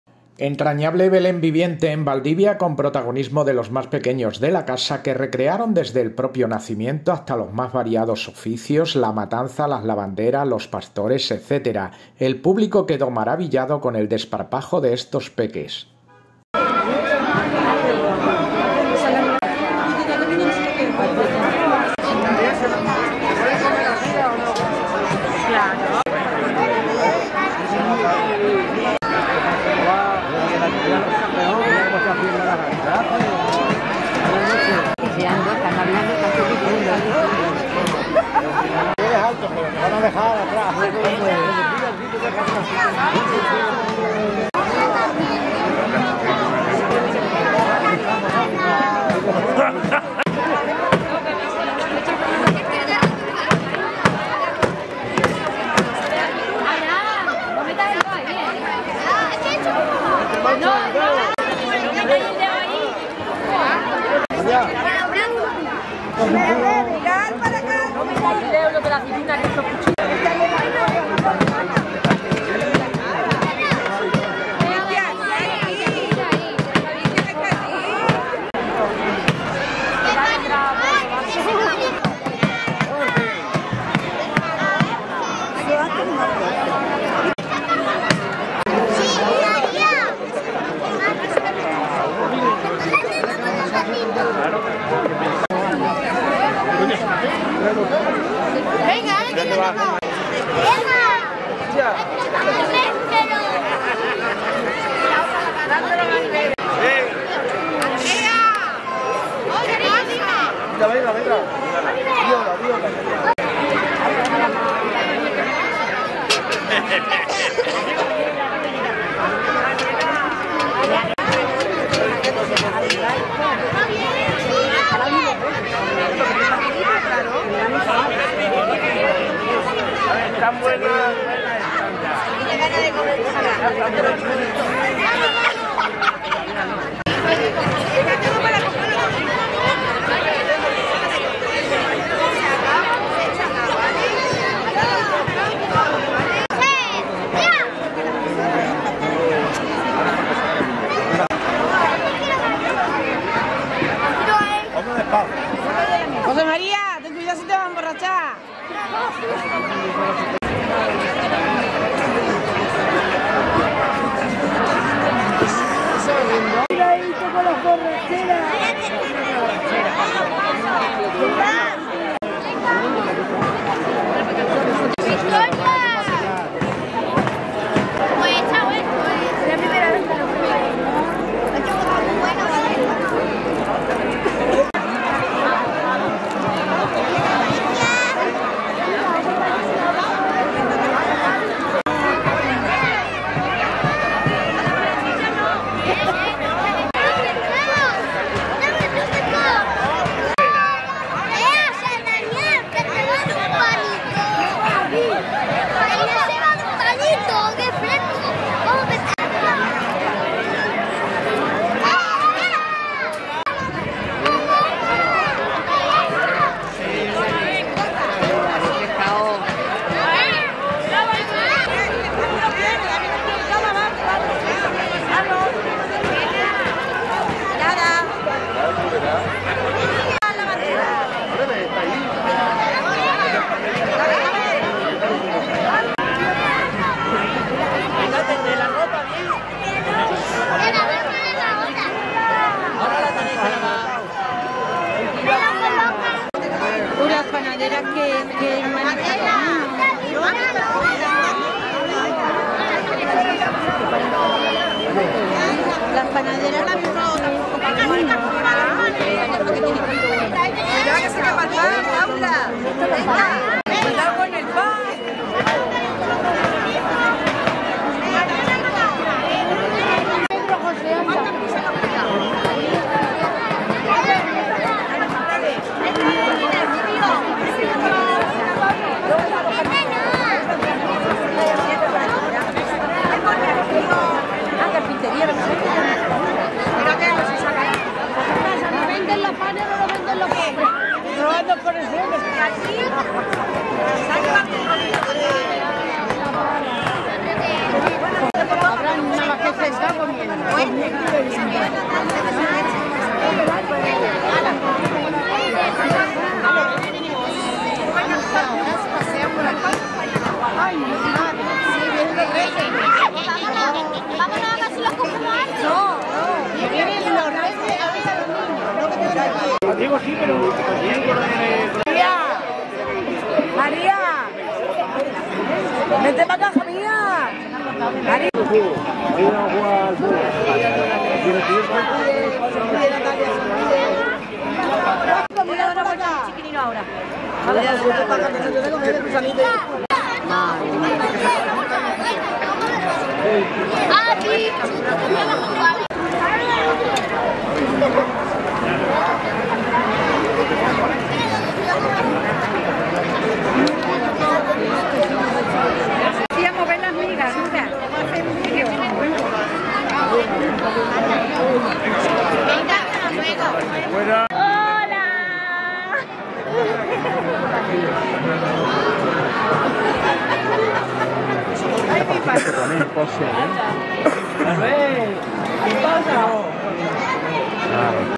Belén viviente Valdivia
Belén viviente en Valdivia. Diciembre de 2022. Canal Nuestra Comarca TV.